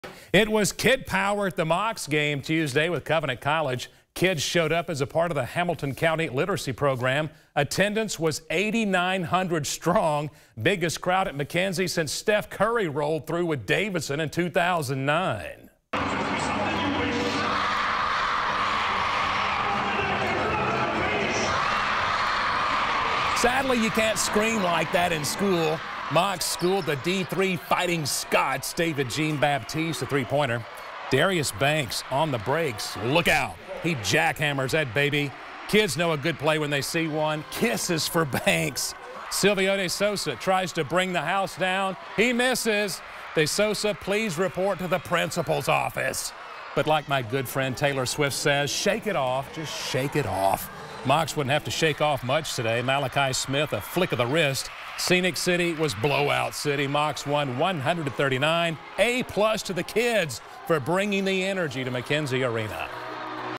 Mocs Feel Kid Power in Beating Covenant College
It was Kid Power at the Mocs game Tuesday with Covenant College.
Kids showed up as apart of the hamilton County literacy program.